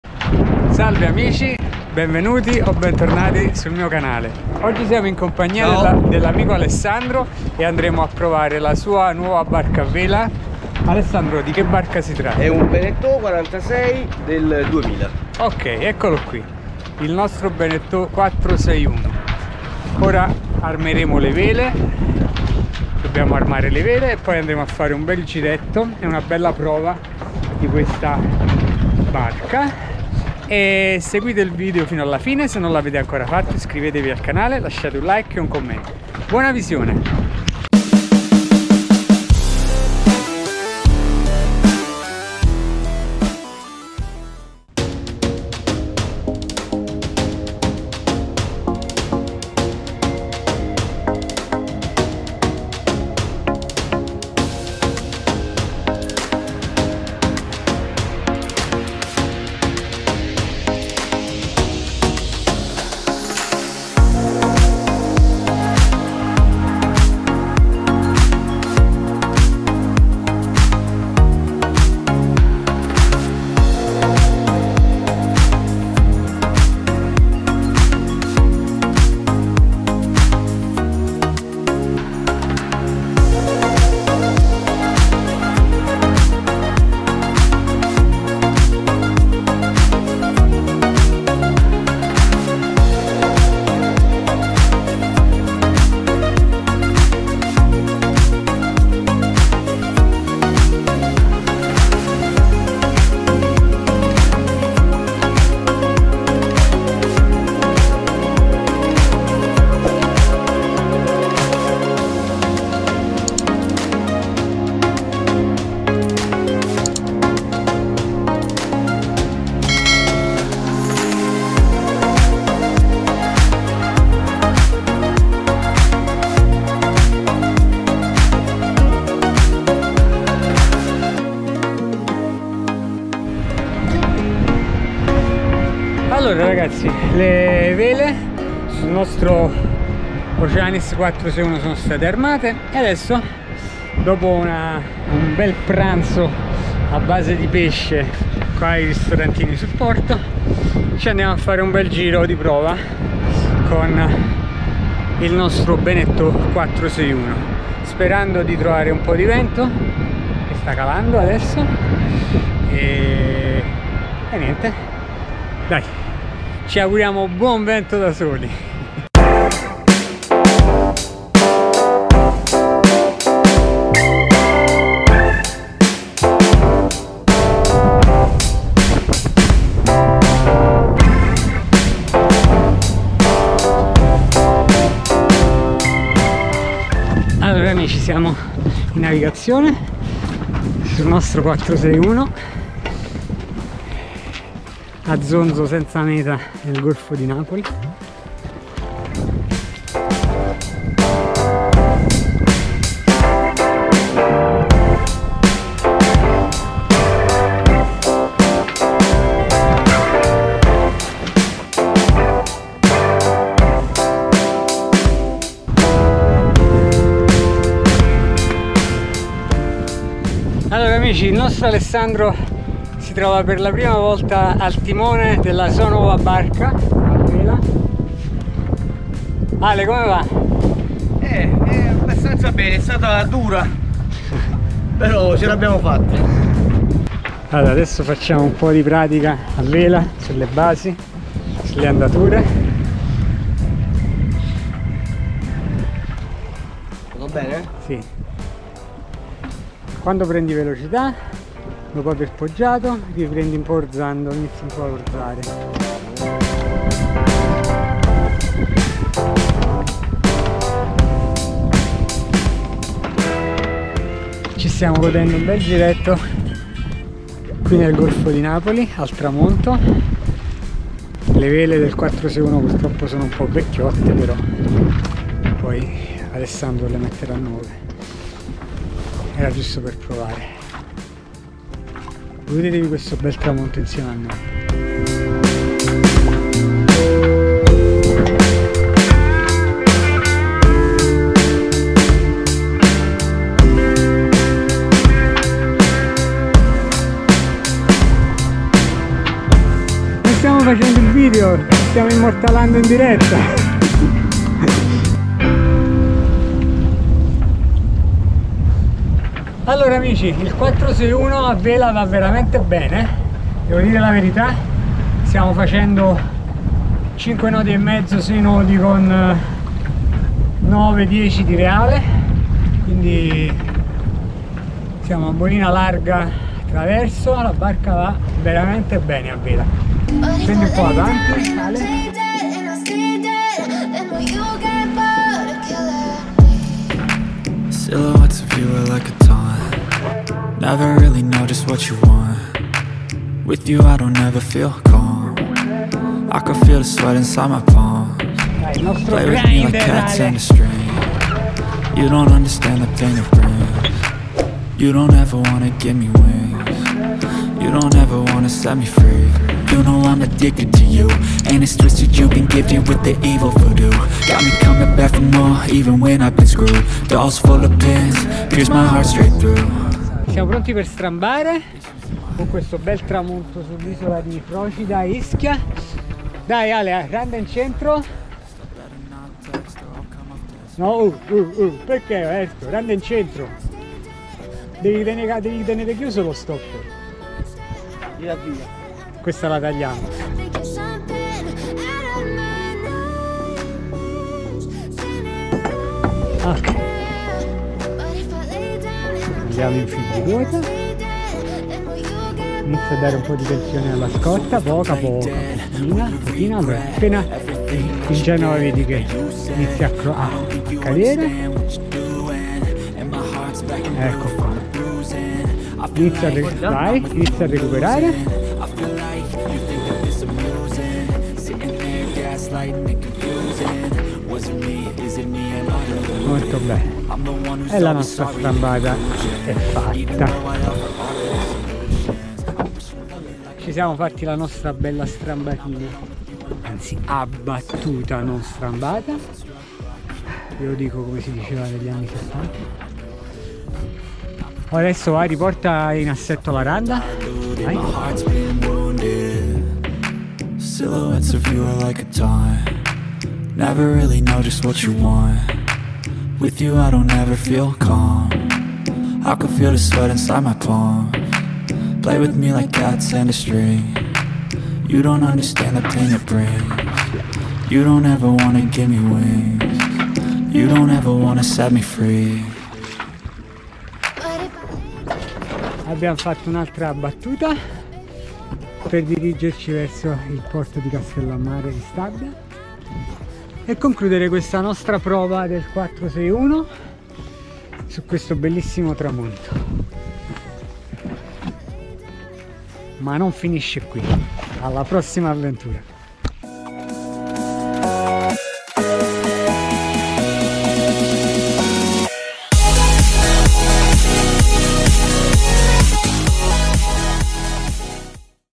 BENETEAU OCEANIS 461 navigare a vela al tramonto nel Golfo di Napoli ⛵
Océanis 461 Barca solida e sicura con un piano velico facile da gestire e molto spazio sottocoperta.
 Si tratta di un modello della Bénéteau che ha riscosso un notevole successo nel charter, merito della comodità sia esterna che interna. Bruce Farr ha progettato una carena che si dimostra equilibrata a vela, dove viene privilegiata la facilità di conduzione e di manovra; ma che garantisce buone prestazioni anche con il motore da 60 hp. Spazioso il pozzetto con un timone a ruota e un comodo accesso alla spiaggetta di poppa; il trasto della randa è sulla tuga dove c’è anche un tientibene semicircolare a protezione di chi manovra all’albero. Oltre alla versione per il noleggio (con 4 cabine e due bagni), gli interni sono disponibili anche con due o tre cabine e due o tre bagni, in tutte si apprezza l’ampio quadrato e la grande cucina a murata.
(fonte bolina)

Dettagli tecnici:
Lunghezza: 13,76m
Larghezza: 4,12m
Pescaggio: 1,75m
Peso: 12,90t
Velatura: 33,70mq
Tipo: Monoscafo

Cantiere: Bénéteau, Francia

Progettista: Bruce Farr